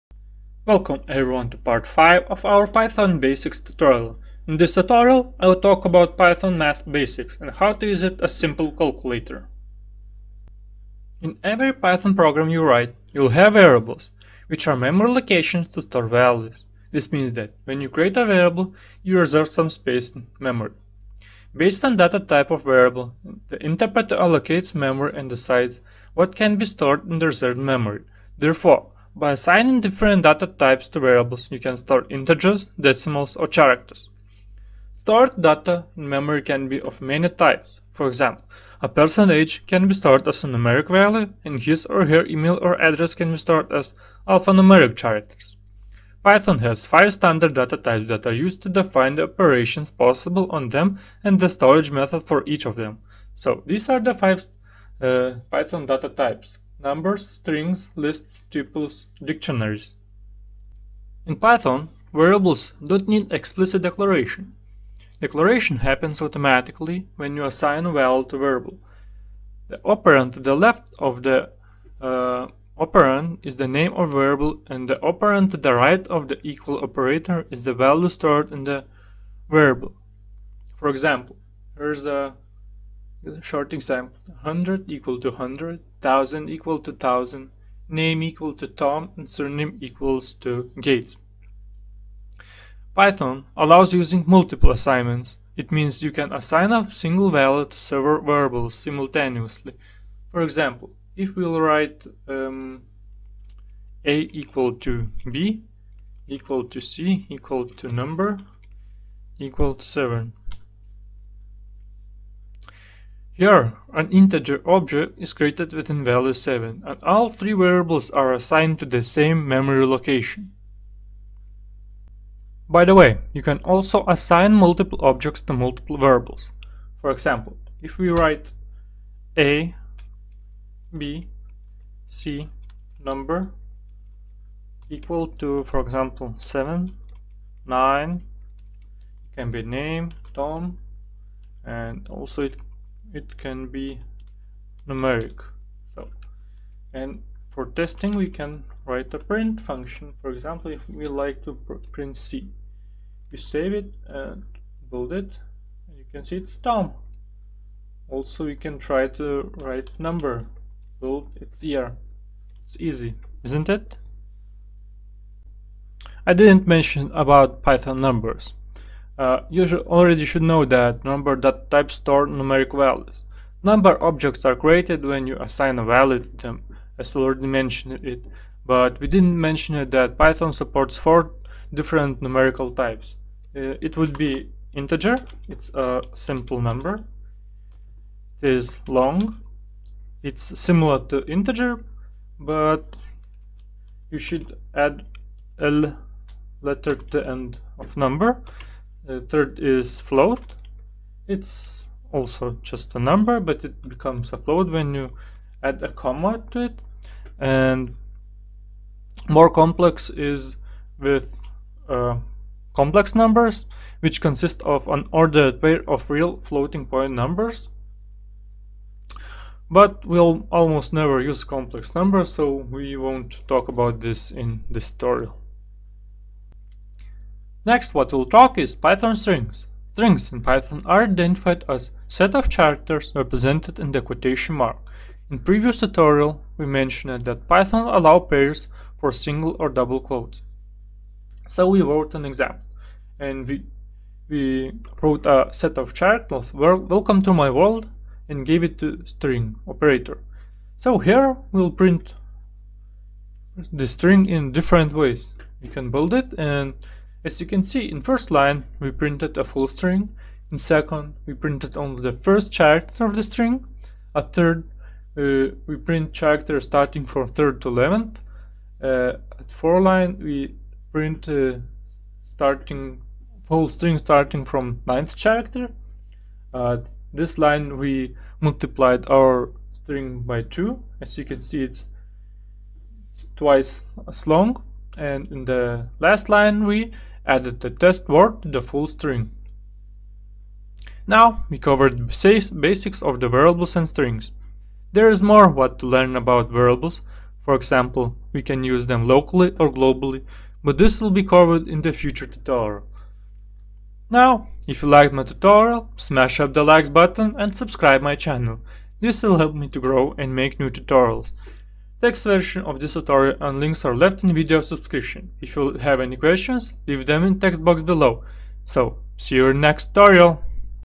#5 Learn to use Python variables and strings
In every Python program you write, you will have variables, which are memory locations to store values. This means that when you create a variable you reserve some space in memory.

Based on the data type of a variable, the interpreter allocates memory and decides what can be stored in the reserved memory. Therefore, by assigning different data types to variables, you can store integers, decimals or characters in these variables.

In Python, variables don't need explicit declaration, declaration happens automatically when you assign a value to a variable. The operand to the left of the = operator is the name of the variable and the operand to the right of the = operator is the value stored in the variable.